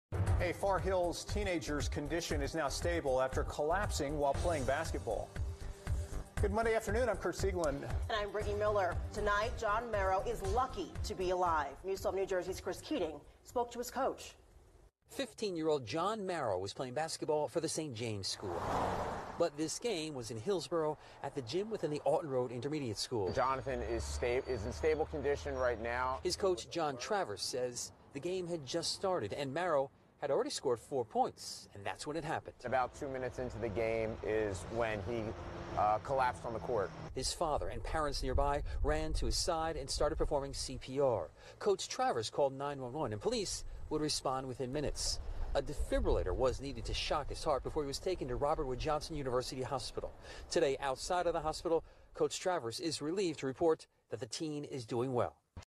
Teen Collapses @ Basketball Game
A 15 year old collapsed during a school basketball game earlier this week.  2 minutes into the game, the player collapsed, and cpr was administered.  EMS had to shock his heart with a defibrillator before rushing him to the hospital.